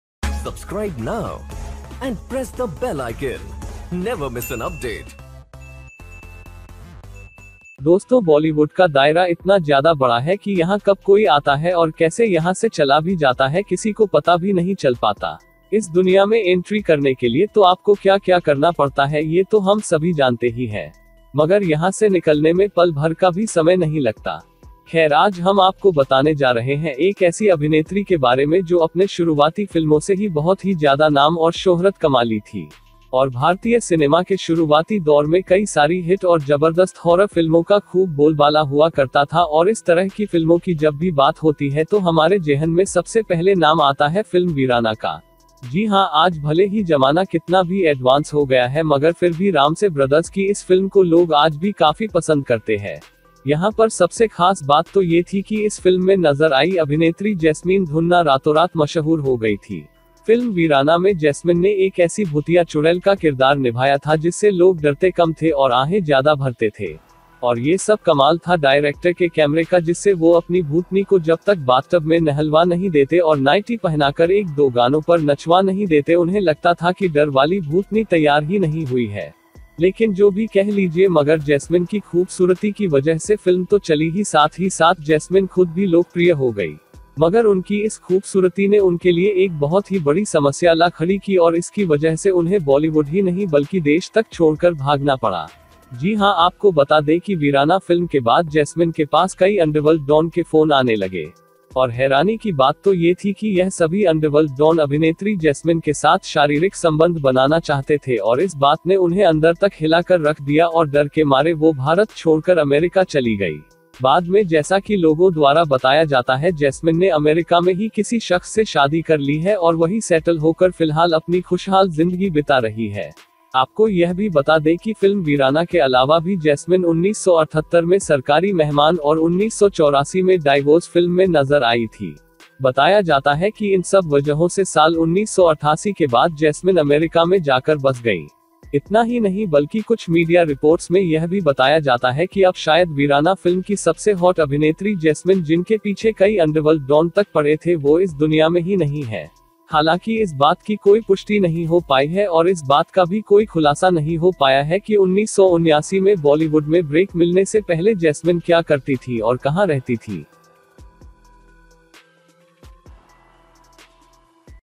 जानिए, अब कहां है वीराना फिल्म की ये खूबसूरत अभिनेत्री | Where is Veerana Film actress Jasmine now?
Writer & Voice: Sonu khan